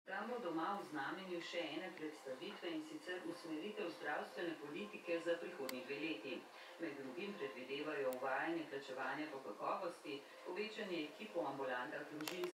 2023-09-13 LJUBLJANA Doma CARNIVORA Felis catus DOMAČA MAČKA (Bunkica)
dan je čudovit, Bunkica pa še kar spi ... lep dan Ti želim Mucica